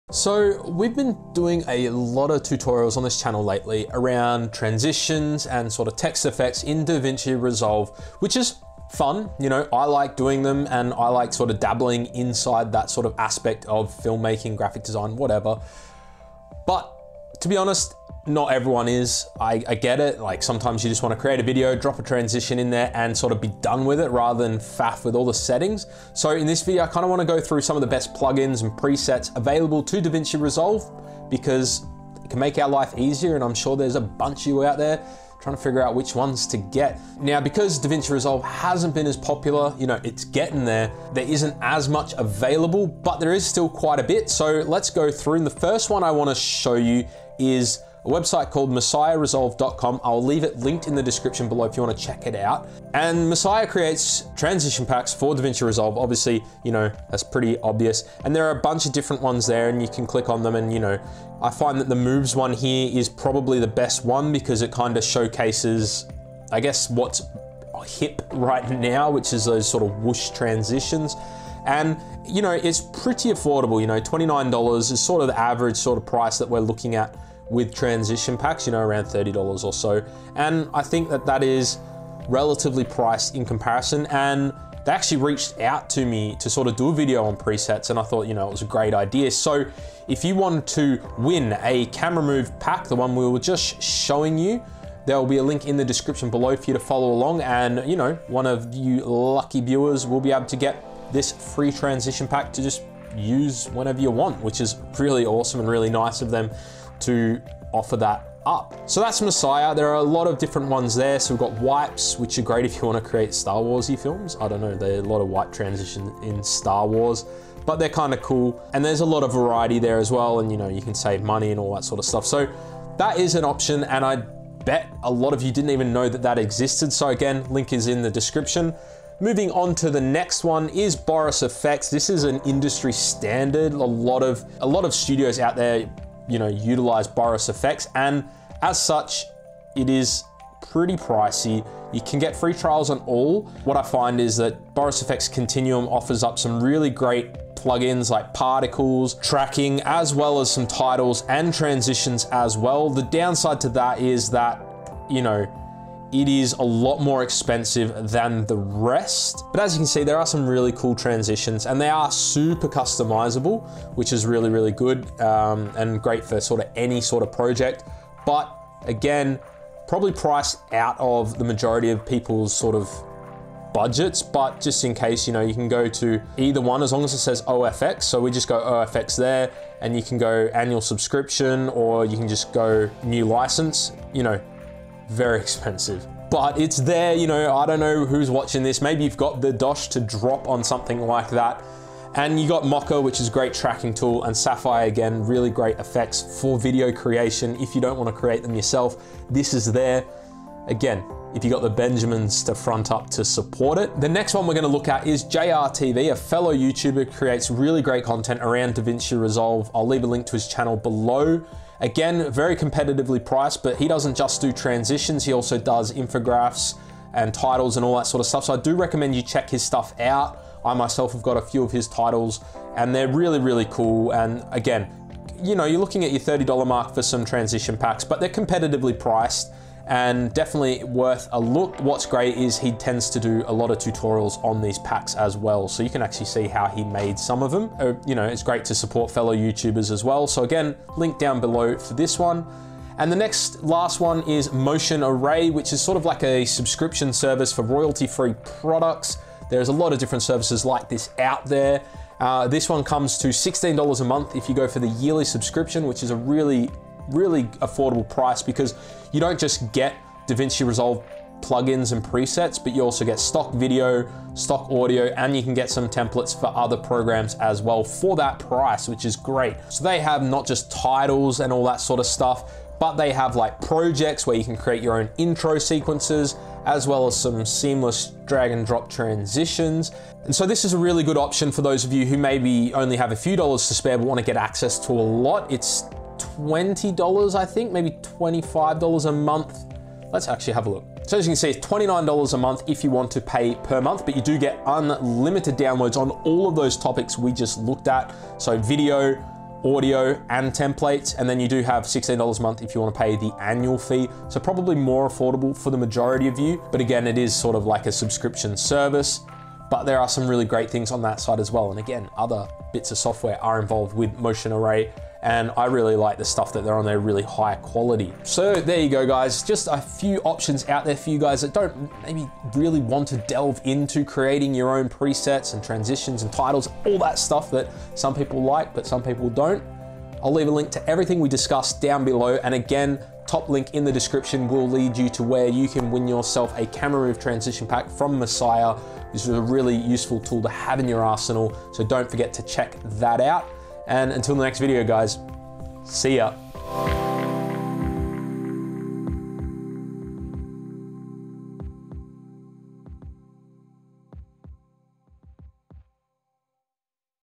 Best Plugins for Davinci Resolve 2019
Check out what I believe to be the best Plugins you can get for Davinci Resolve 16!  The biggest downside in my opinion to Davinci Resolve is the fact that there isn't too many Plugins available compared to the competition.
But after doing a lot of searching these are the ones I think you need to know about!